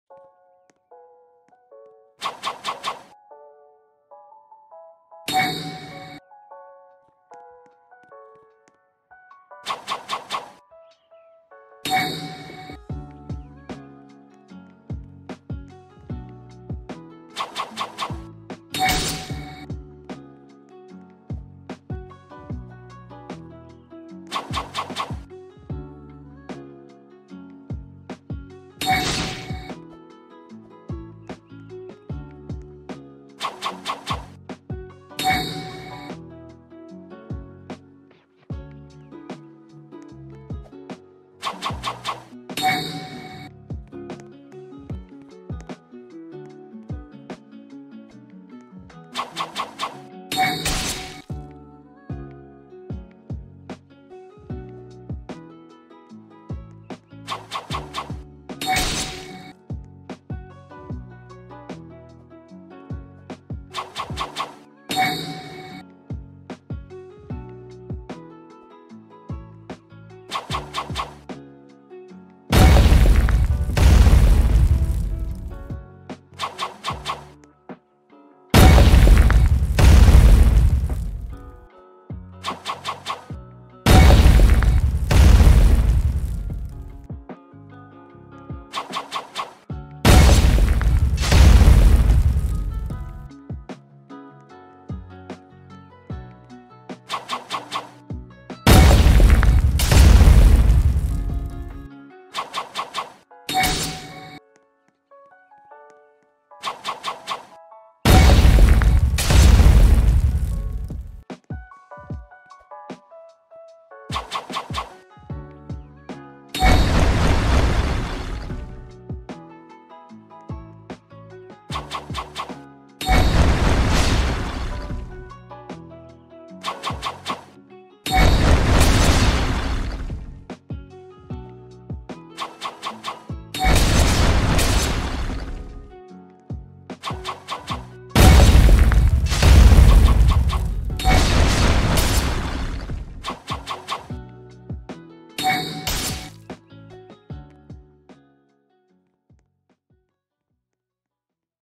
Naruto Earth Element Showcase - Roblox Studio
The showcase of an element of a Naruto NON adventure game project.